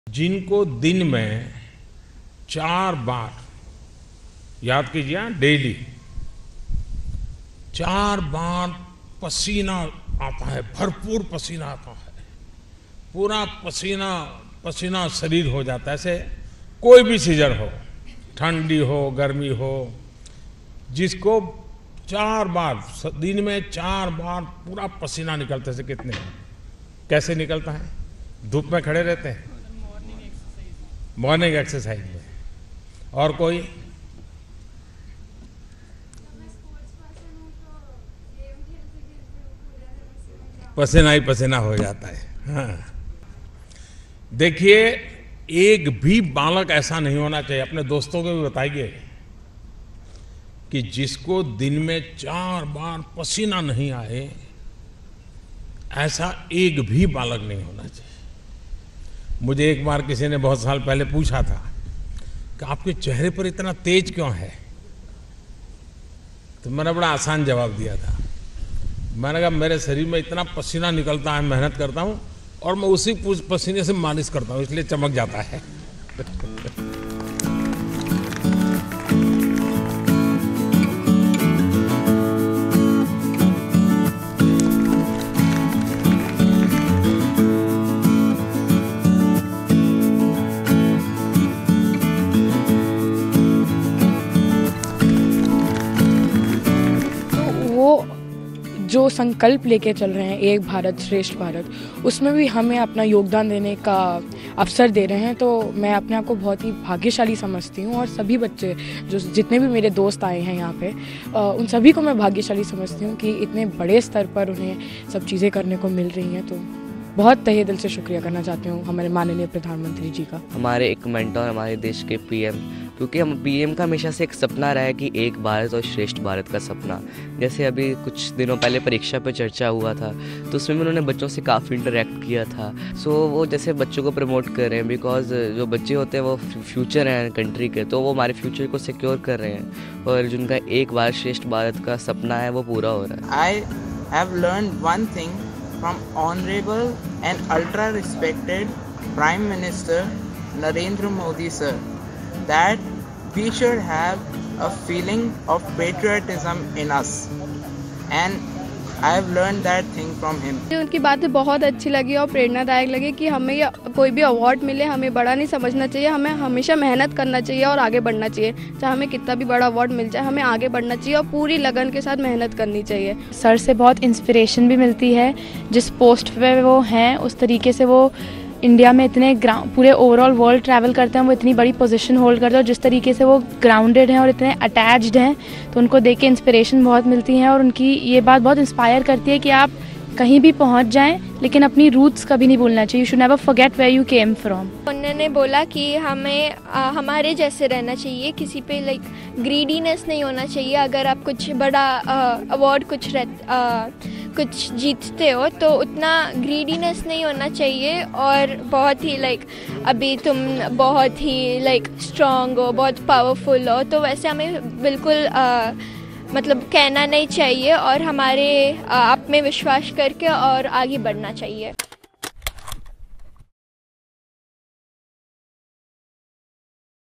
Rashtriya Bal Puraskar विजेता बच्चों को PM Modi ने बताया चेहरे की चमक का राज I Kadak
PM Narendra Modi ने Rashtriya Bal Puraskar विजेता बच्चों से मुलाकात की. पीएम ने बच्चों की बहादुरी की सराहना की. इस मौके पर उन्होंने बच्चों से ढेर सारी बातें कीं. पीएम ने कहा कि युवा साथियों के ऐसे साहसिक कार्यों के बारे में सुनने से उन्हें प्रेरणा और ऊर्जा मिलती है.